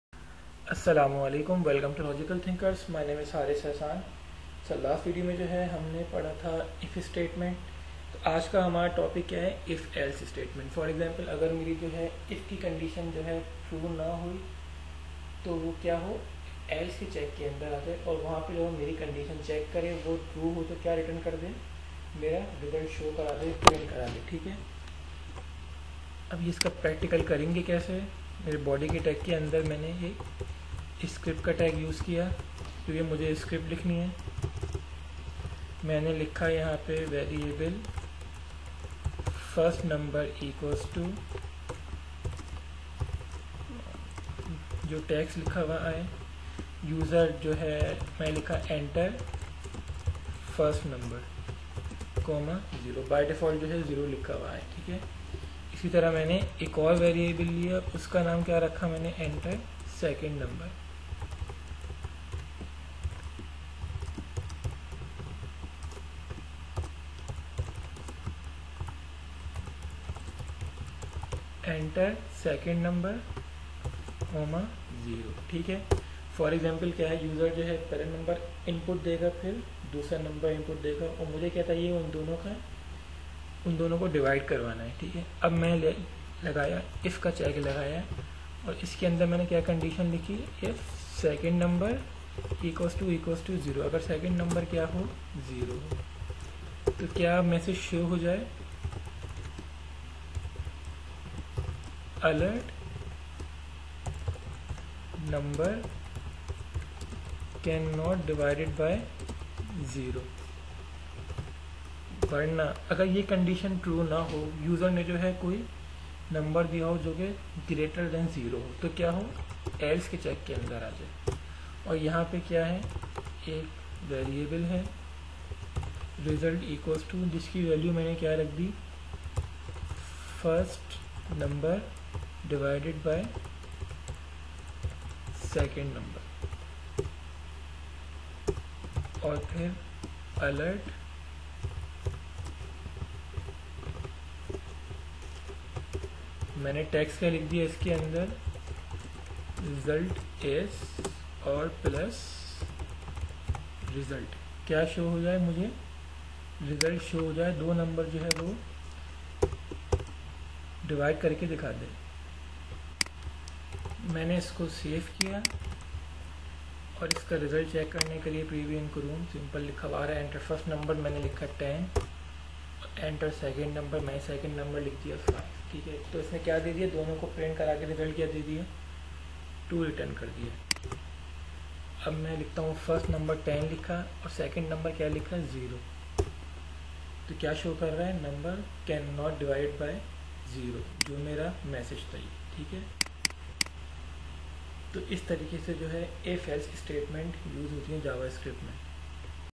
Part:26 (if-else Statement) Java Script tutorial for Beginners in Urdu/Hindi by Haris Ahsan
if-else Statement
The if statement specifies a block of statement to be executed when the condition in the if statement is true. However, sometimes it is required to define a block of statements to be executed when a condition is evaluated to false. This is done using the if-else
statement.